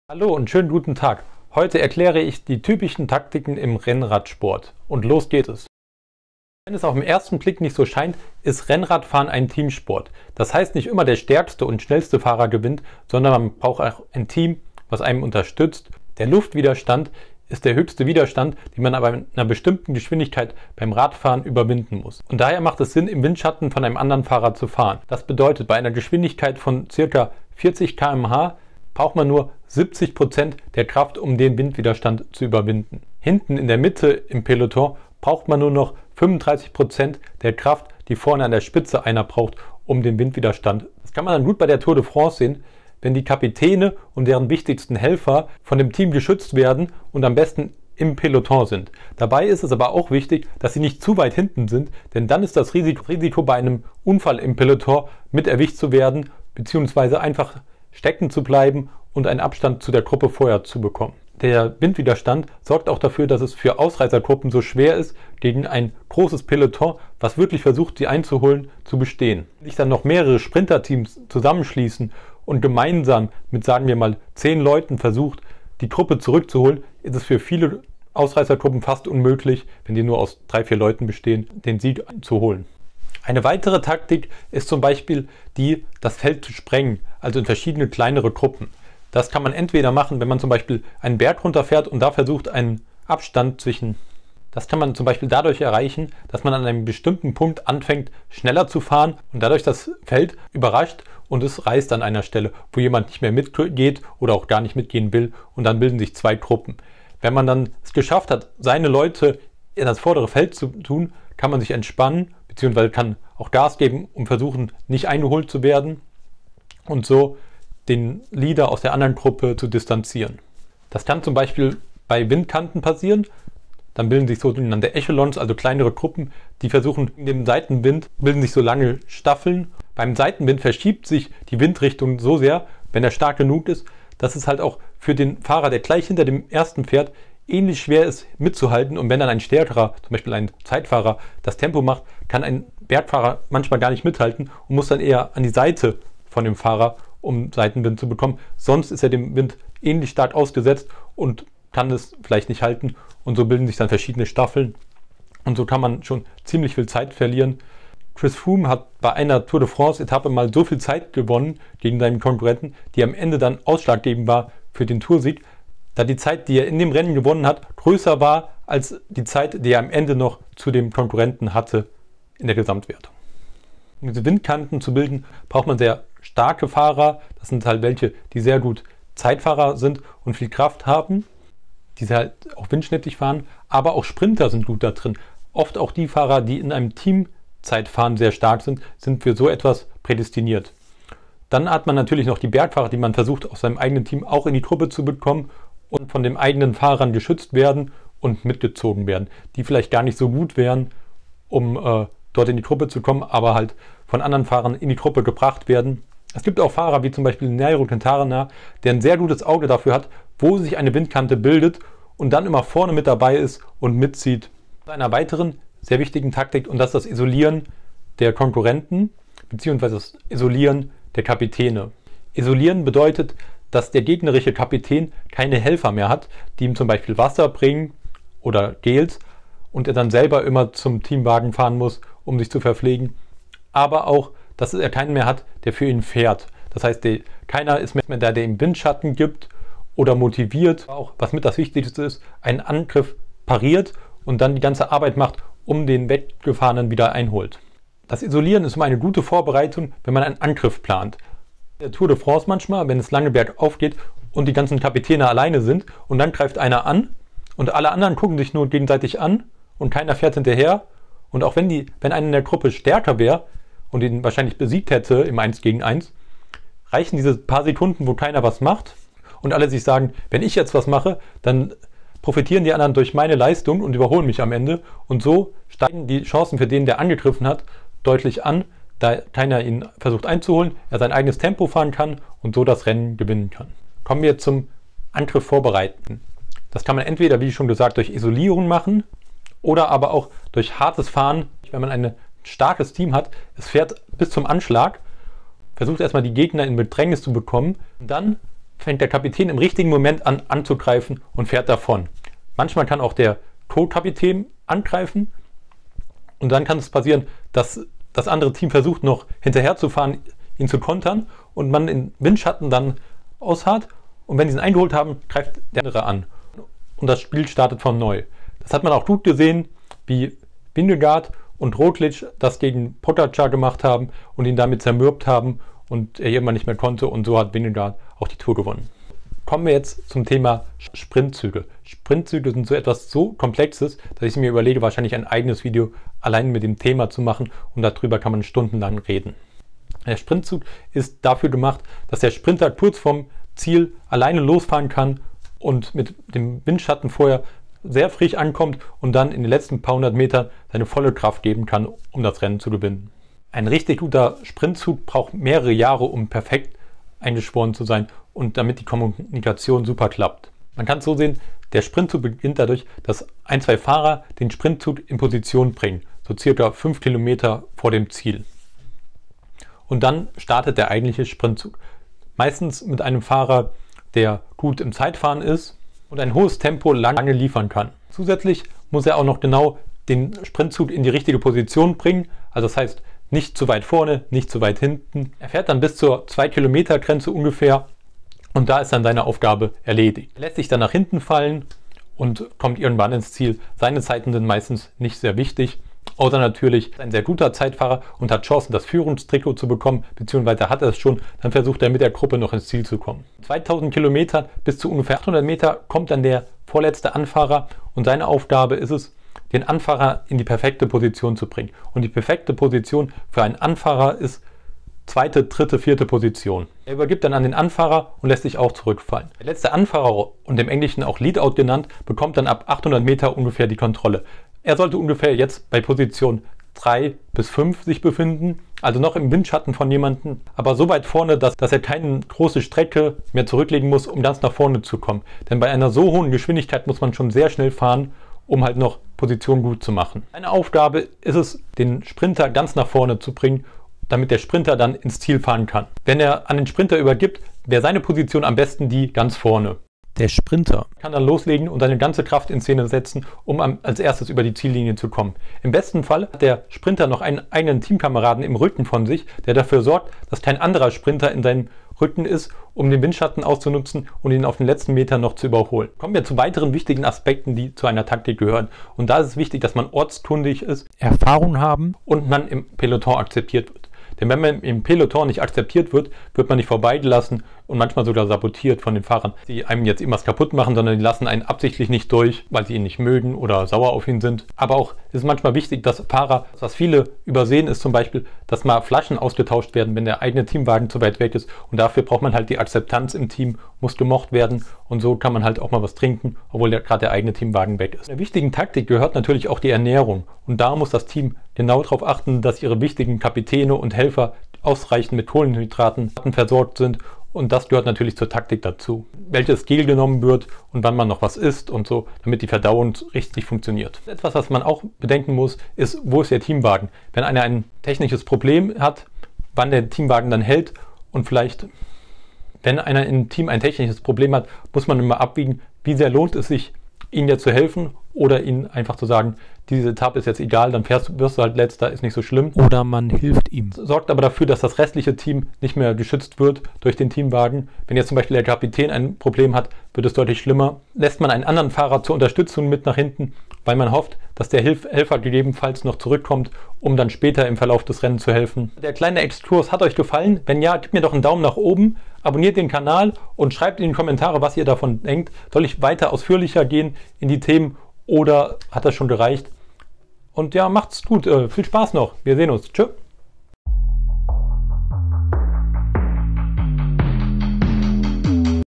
Die verschiedenen Rennradtaktiken schnell erklärt
Ich beschreibe verschienene Strategien die man bei einem Radrennen verwernden kann um ein Rennen zu gewinnen.